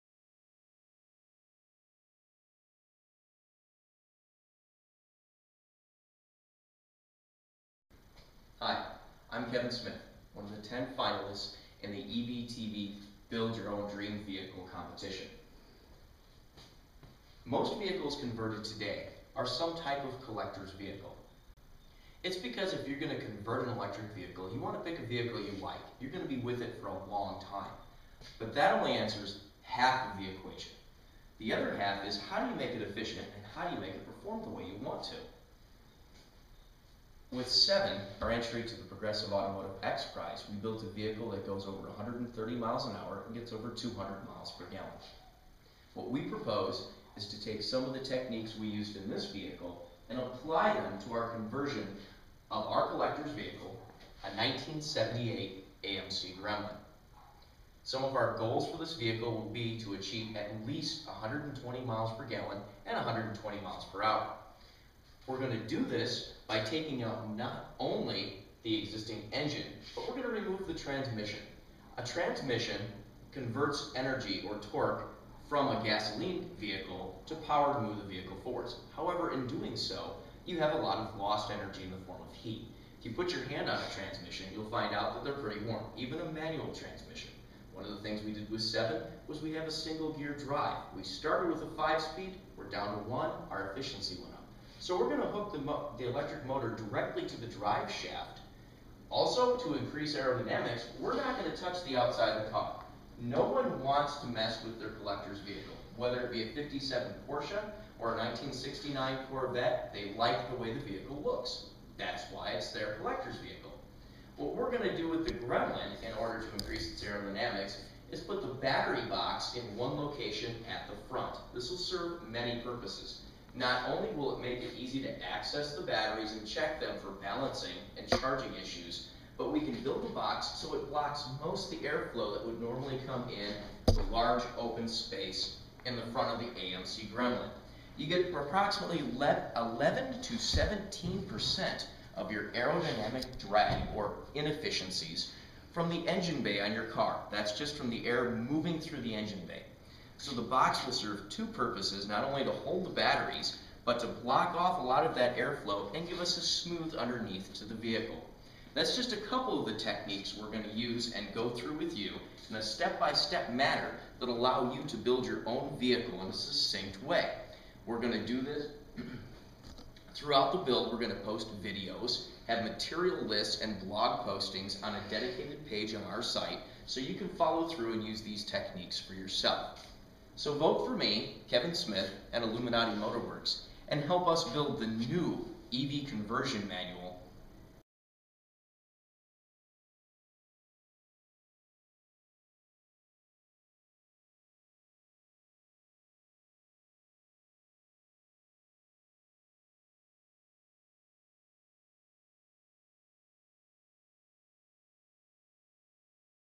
IMW: 2011 EVTV Build Your Dream EV Contest Proposal
Kevin Smith explains his plan for Illuminati Motor Works to convert a 1978 AMC Gremlin to an electric vehicle if he wins the EVTV Build Your Dream EV Contest. Please vote for Kevin and Illuminati Motor Works